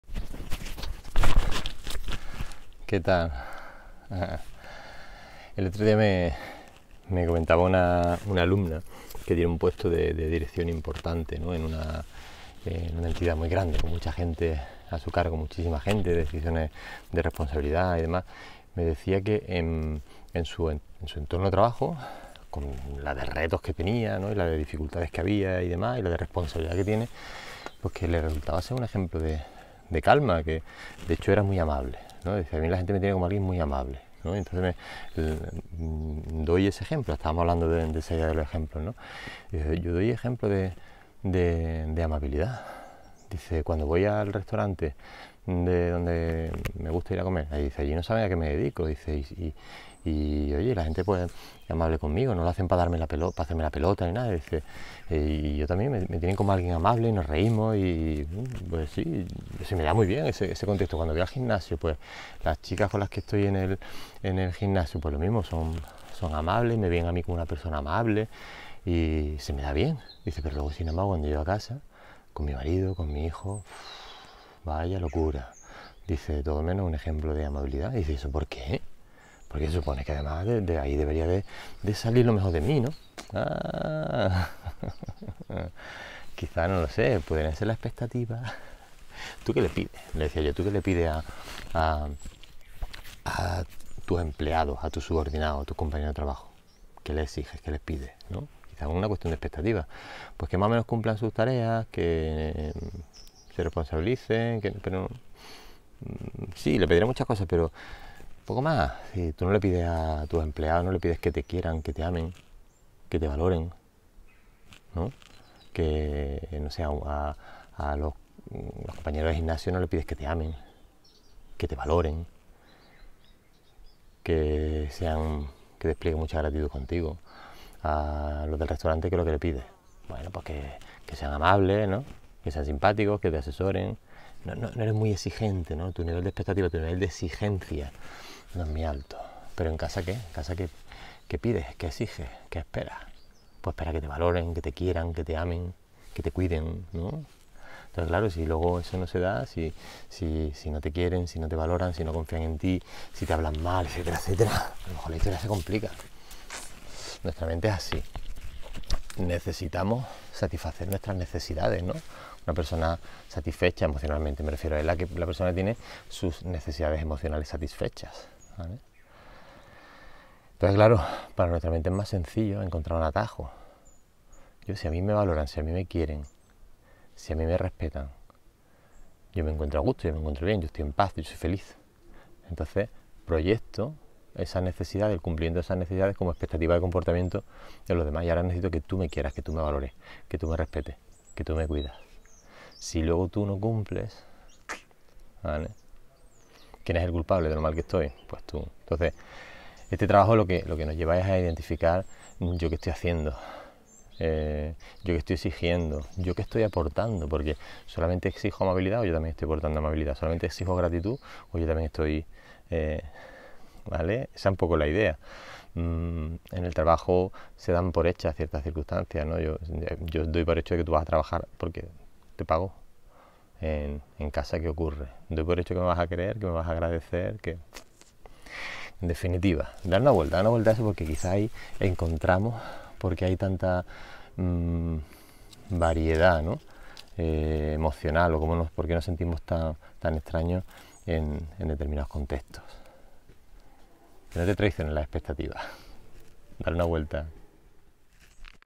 [71 de 365] demasiados enfados en casa?
¿Tienes el mismo número de situaciones de conflicto en casa que en otros contextos? Si te resulta más sencillo ser un ejemplo de calma fuera de casa que en casa, igual te interesa darle un vistazo a este vídeo. ¿me acompañas?